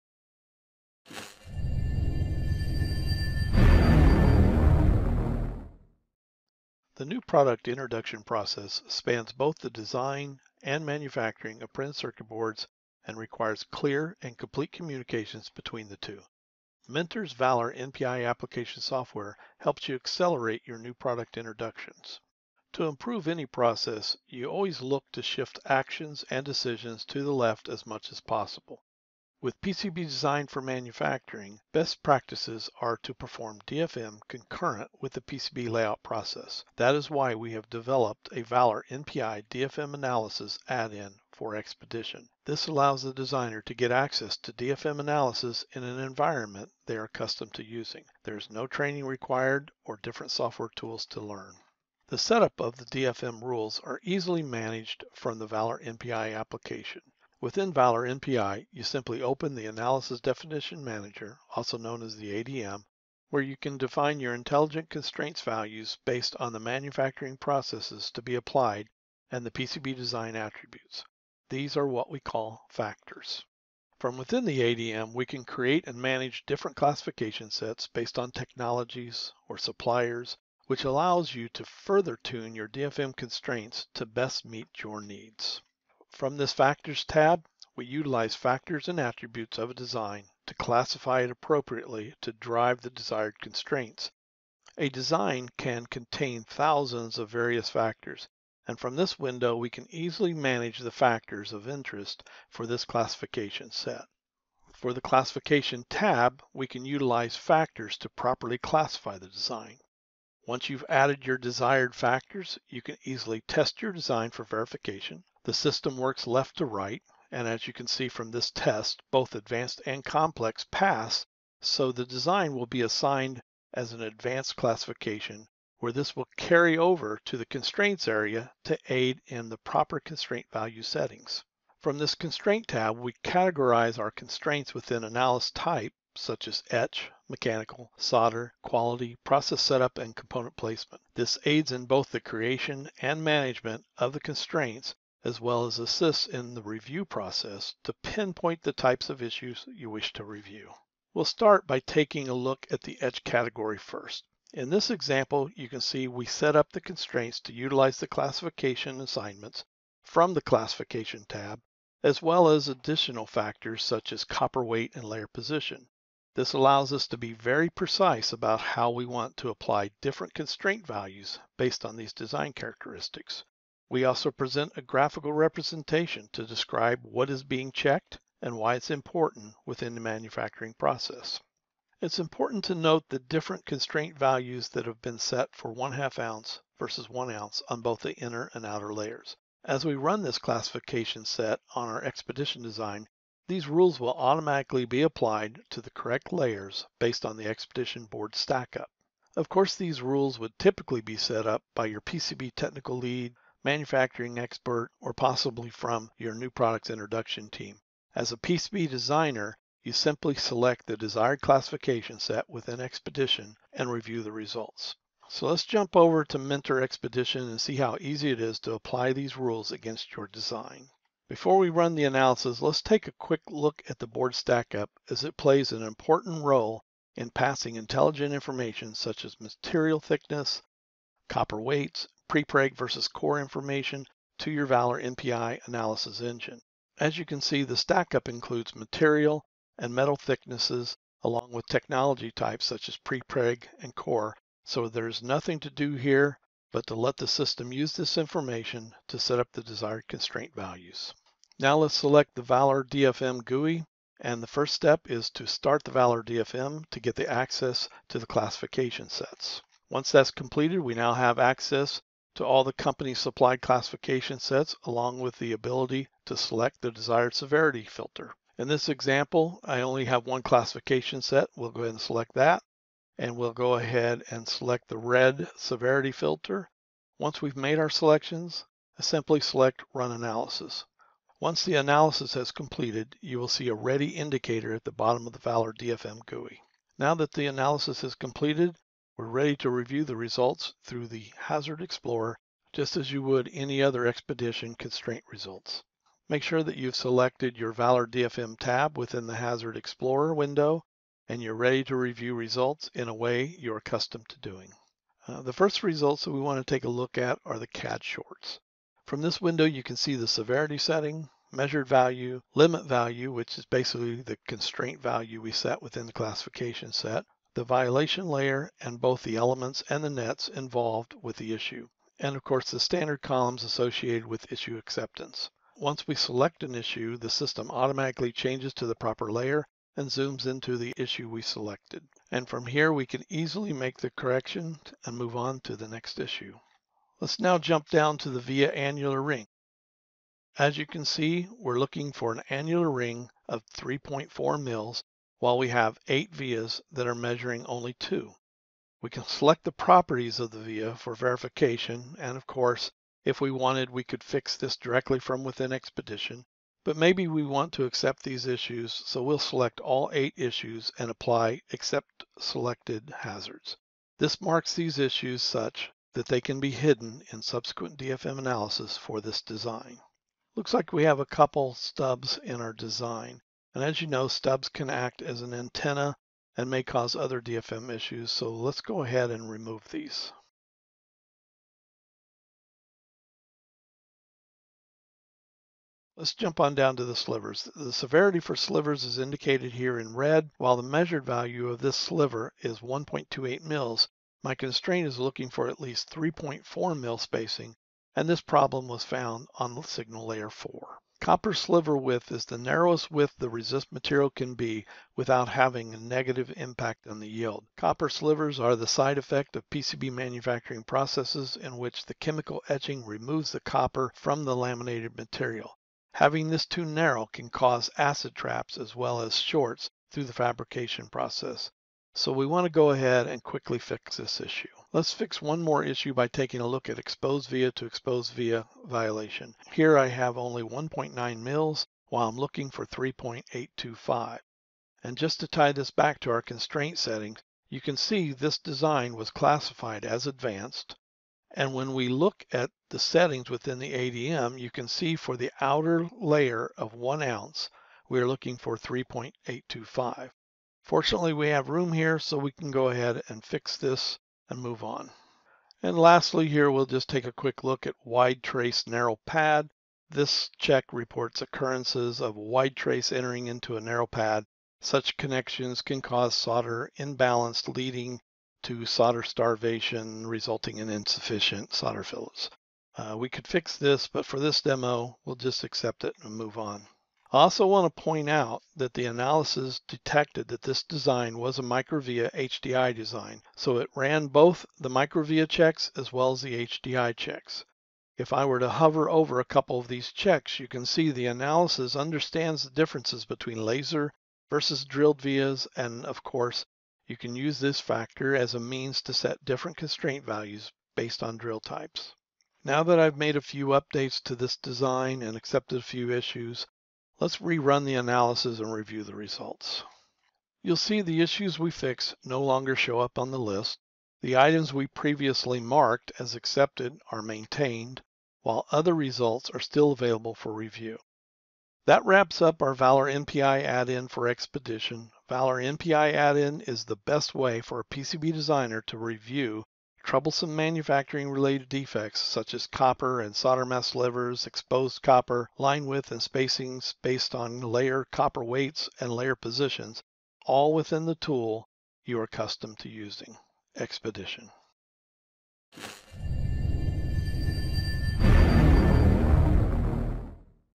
Intelligent and Integrated PCB DFM
This video demonstrates how easy it is to manage PCB manufacturing constraints such as material thickness, copper weighting, and prepreg vs core information during layout in your design tool.